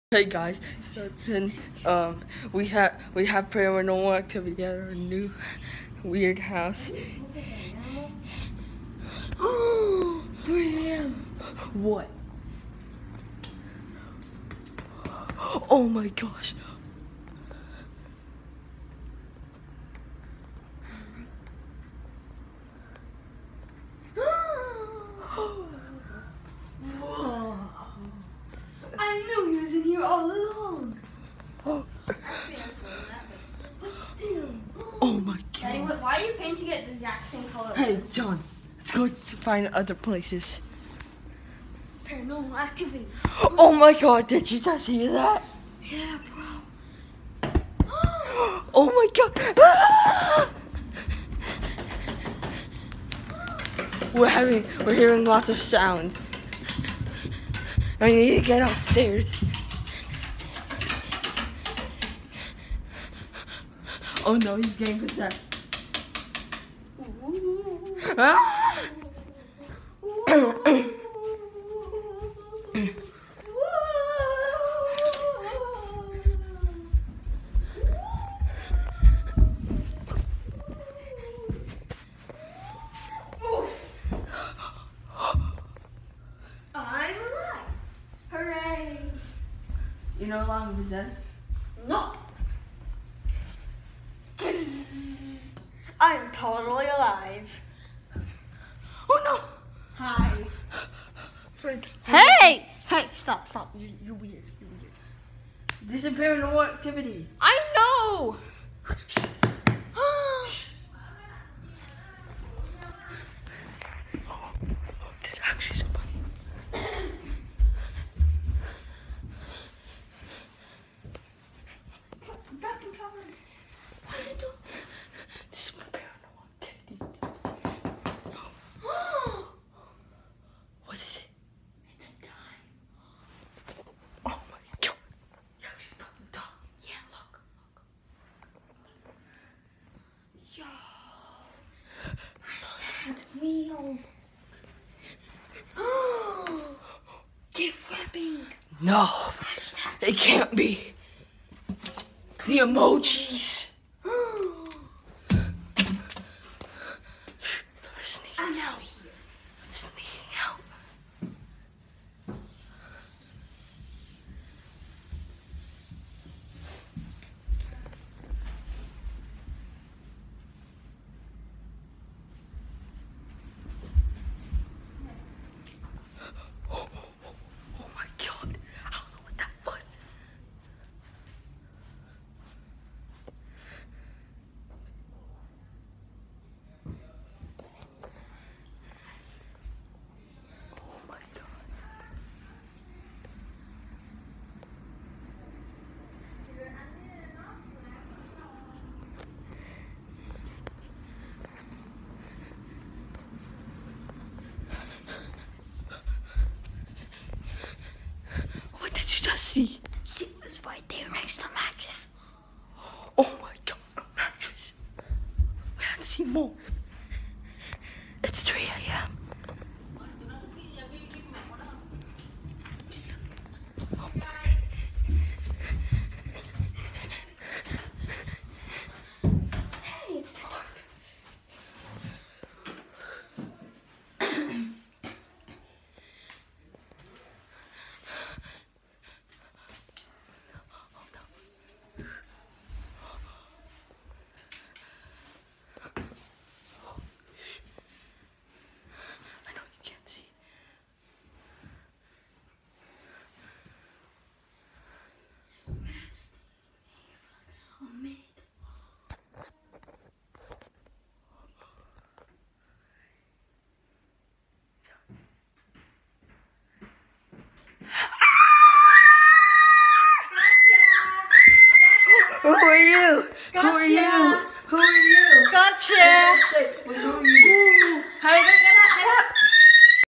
Dumb Kids doing Paranormal Activity (super fake)
Hey guys I haven’t posted in a while and it’s because that we are moving to a new house and it gets really busy so then I will try and upload more
Enjoy the Funny video